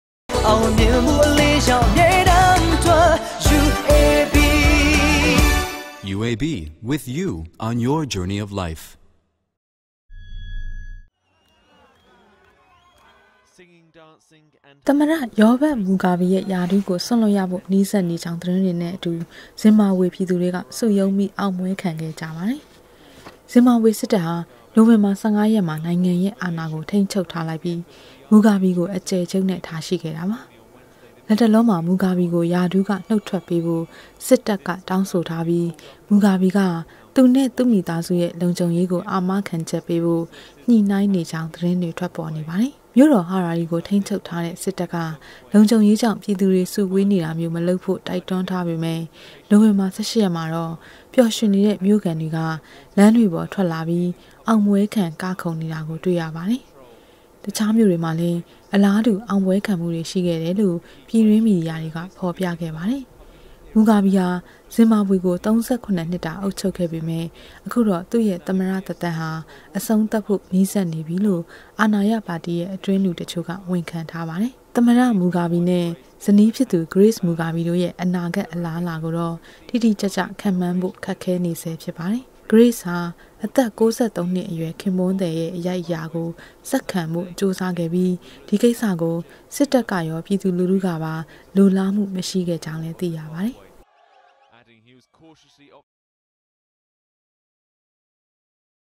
သမၼတ မူဂါဘီ ရာထူးစြန္႔ရႏုိင္ေၾကာင္း သတင္းေတြေၾကာင့္ ဇင္ဘာေဘြ ျပည္သူေတြ ေအာင္ပြဲခံ
သမၼတ ေရာဘတ္ မူဂါဘီရဲ႕ ရာထူးကို စြန္႔လႊတ္ရဖို႔ နီးစပ္ေနေၾကာင္း သတင္းေတြနဲ႔အတူ ဇင္ဘာေဘြ ျပည္သူေတြ စု႐ံုးၿပီး ေအာင္ပြဲခံခဲ့ၾကပါတယ္။

ဇင္ဘာေဘြ စစ္တပ္ဟာ ႏိုဝင္ဘာ ၁၅ ရက္မွာ ႏုိင္ငံရဲ႕ အာဏာကို ထိန္းခ်ဳပ္ထားလုိက္ၿပီး မူဂါဘီကို အက်ယ္ခ်ဳပ္နဲ႔ ထားရွိပါတယ္။ လတ္တေလာမွာ မူဂါဘီကို ရာထူးက ႏုတ္ထြက္ေပးဖုိ႔ စစ္တပ္က ေတာင္းဆိုထားၿပီး မူဂါဘီက သူနဲ႔ သူ႔မိသားစုရဲ႕ လံုၿခံဳေရးကို အာမခံခ်က္ေပးဖို႔ ညႇိႏိႈင္းေနေၾကာင္း သတင္းေတြ ထြက္ေပၚေနပါတယ္။

ၿမိဳ႕ေတာ္ ဟာရာရီကို ထိန္းခ်ဳပ္ထားတဲ့ စစ္တပ္က လံုၿခံဳေရးေၾကာင့္ ျပည္သူေတြ စုေဝးေနတာမ်ိဳး မလုပ္ဖို႔ တိုက္တြန္းထားေပမယ့္ ႏိုဝင္ဘာ ၁၈ ရက္မွာေတာ့ ေပ်ာ္ရႊင္ေနတဲ့ ၿမိဳ႕ခံေတြက လမ္းေတြေပၚ ထြက္လာၿပီး ေအာင္ပြဲခံ ကခုန္ေနတာကို ေတြ႕ရပါတယ္။ တျခားၿမိဳ႕ေတြမွာလည္း အလားတူ ေအာင္ပြဲခံမႈေတြ ရွိခဲ့တယ္လုိ႔ ျပည္တြင္း မီဒီယာေတြက ေဖာ္ျပခဲ့ပါတယ္။ မူဂါဘီဟာ ဇင္ဘာေဘြကို ၃၇ ႏွစ္တာ အုပ္ခ်ဳပ္ခဲ့ေပမယ့္ အခုေတာ့ သူ႔ရဲ႕ သမၼတသက္တမ္းဟာ အဆံုးသတ္ဖုိ႔ နီးစပ္ေနၿပီလုိ႔ အာဏာရပါတီရဲ႕ အတြင္းလူ တခ်ိဳ႕က ဝန္ခံထားပါတယ္။

သမၼတ မူဂါဘီနဲ႔ ဇနီးျဖစ္သူ ဂေရ႕စ္မူဂါဘီတို႔ရဲ႕ အနာဂတ္ အလားအလာကိုေတာ့ တိတိက်က် ခန္႔မွန္းဖို႔ ခက္ခဲေနဆဲ ျဖစ္ပါတယ္။ ဂေရ႕စ္ဟာ အသက္ ၉၃ ႏွစ္အရြယ္ ခင္ပြန္းသည္ရဲ႕ အ႐ိုက္အရာကို ဆက္ခံဖုိ႔ ႀကိဳးစားခဲ့ၿပီး ဒီကိစၥကို စစ္တပ္ကေရာ၊ ျပည္သူလူထုကပါ လိုလားမႈ မရွိခဲ့ေၾကာင္းလည္း သိရပါတယ္။